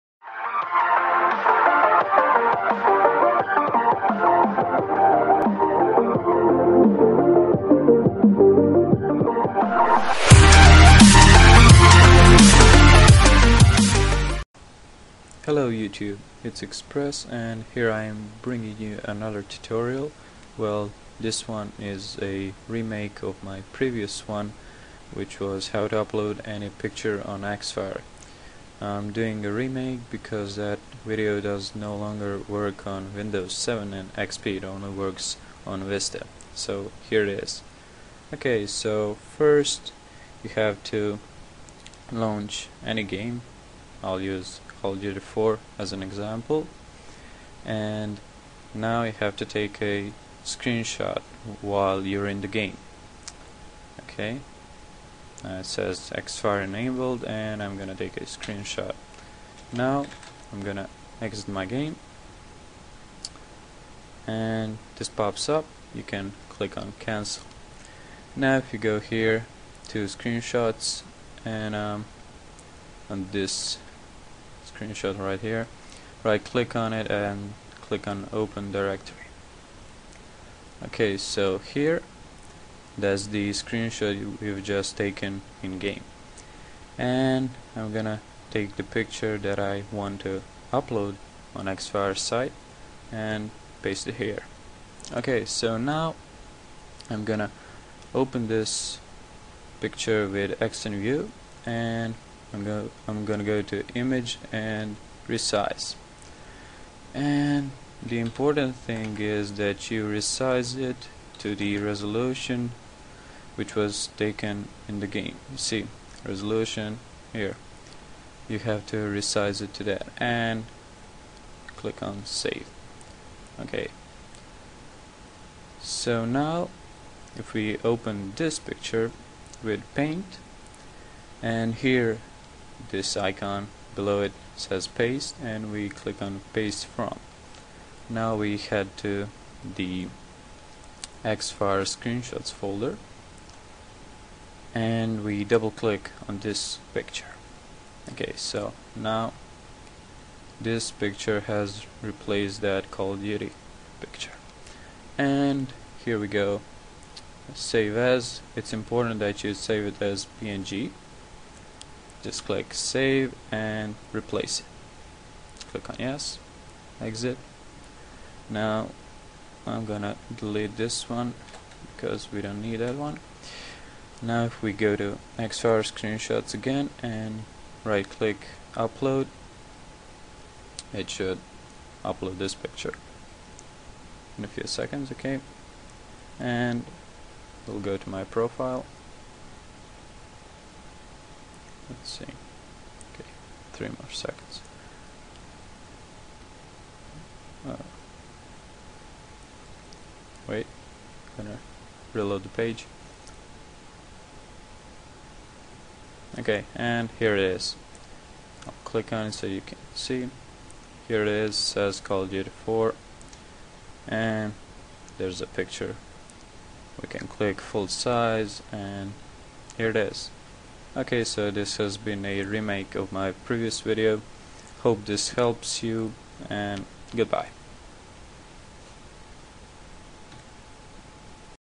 How To Upload Any Screenshot On Xfire by: expresszor (WORKS 2011)
Hello, this a remake of my previous tutorial - this works as you can clearly see...